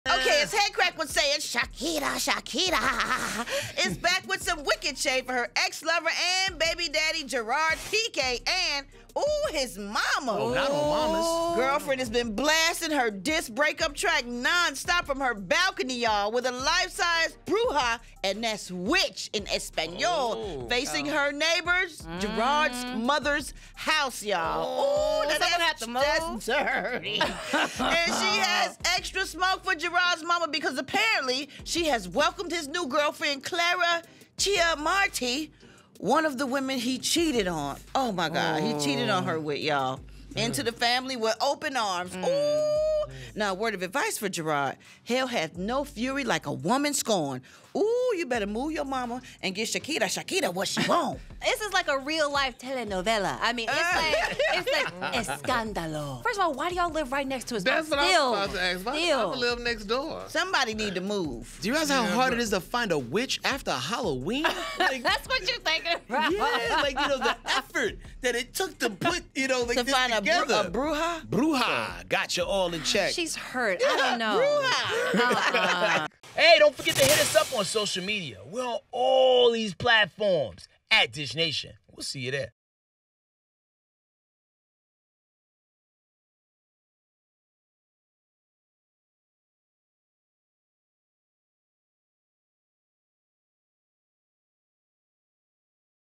Shakira's Breakup Track Is On Repeat & She Is Trolling Her Ex Gerard Pique's Mom To Boot!
Shakira keeps blasting her new diss track at her neighbor who happens to be Gerard's mama! The singer also had a witch figurine facing her neighbor in a slightly veiled attempt to let her feelings be know! MelissaKnowles just wants to know why folks are living next to their parents in the first place!